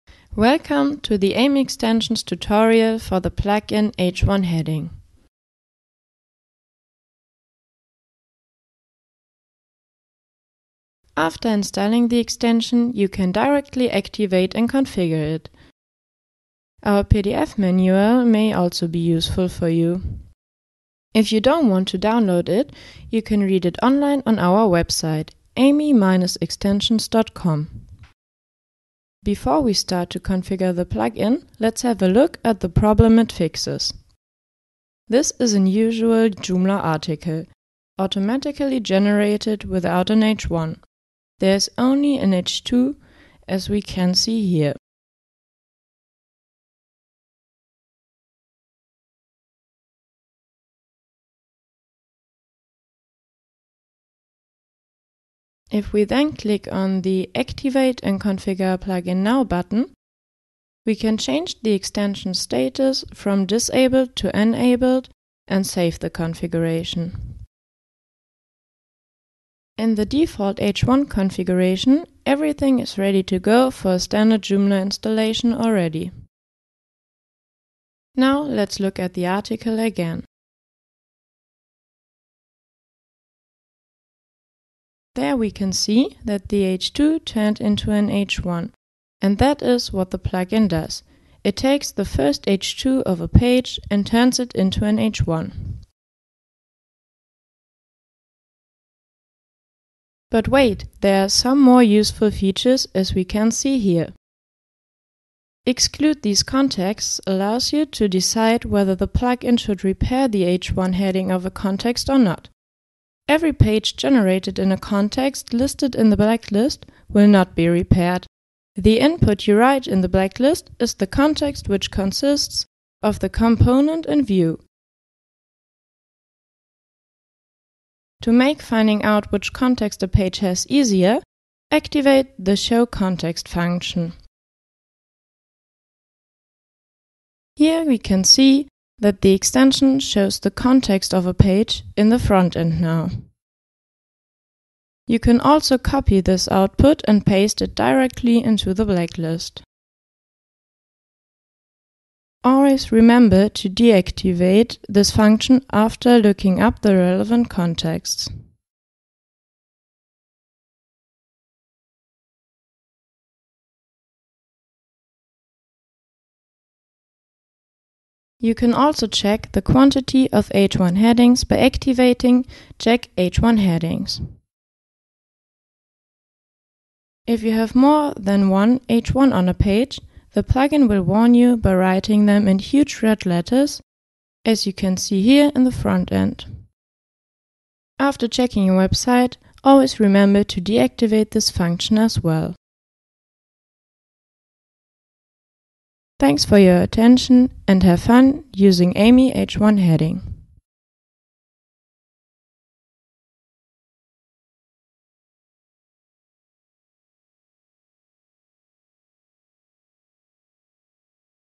Aimy H1 Heading for Joomla! Websites
This video tutorial introduces you to Aimy H1 Heading - a Joomla! extension to easily repair any missing H1 headings of your website.